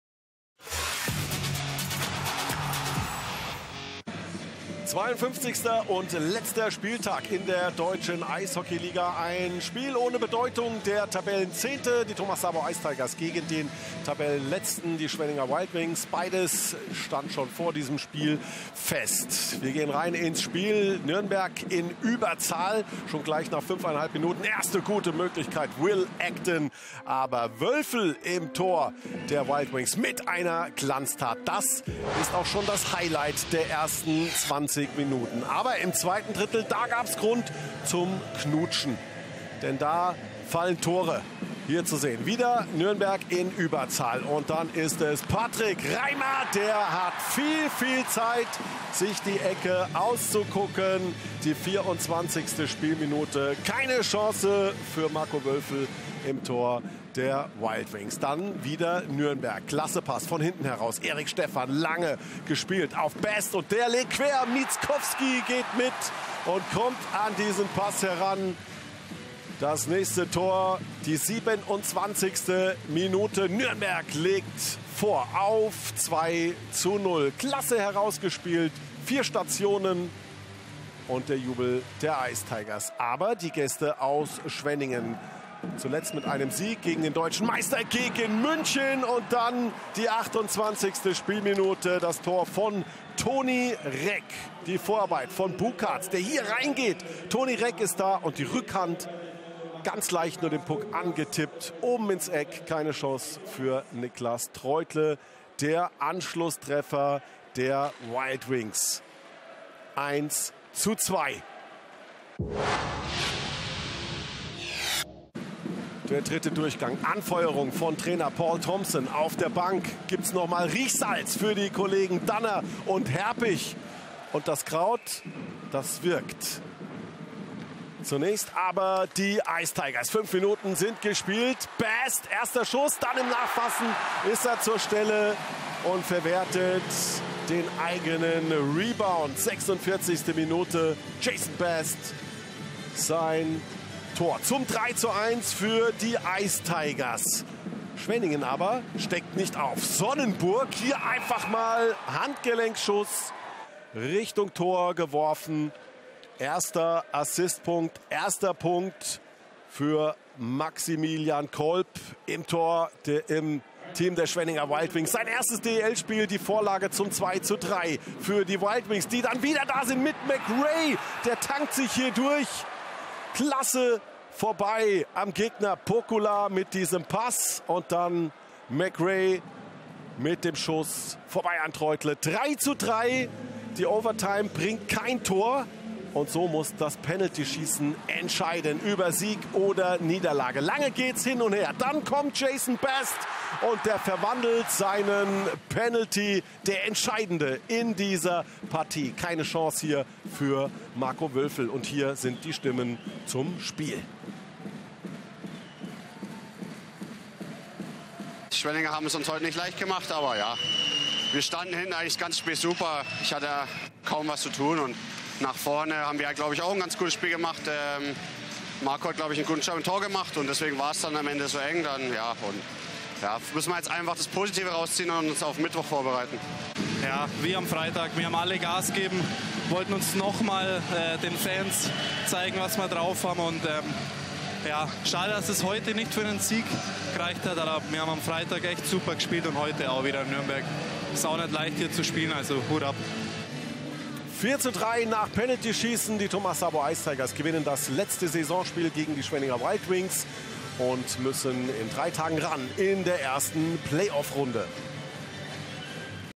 Thomas Sabo Ice Tigers - Schwenninger Wild Wings | 52. Spieltag, 18/19 | MAGENTA SPORT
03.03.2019, 52. Spieltag der Saison 2018/2019. Die Thomas Sabo Ice Tigers ringen die Schwenniner Wild Wings im Shootout nieder. Den entscheidenden Treffer erzielt Jason Bast. In der regulären Spielzeit liegen die Hausherren bereits 3:1 in Führung, kassieren in den letzten acht Minuten aber noch zwei Gegentore durch Kyle Sonnenburg (53.) und Philip McRae (56.).

Aufstellungen:
Thomas Sabo Ice Tigers:
Daniel Weiss, Leonhard Pföderl, Patrick Reimer, Tim Bender, Thomas Gilbert, Niklas Treutle

Schwenninger Wild Wings:
Mirko Höfflin, Ville Korhonen, Markus Poukkula, Tobias Wörle, Jussi Timonen, Marco Wölfl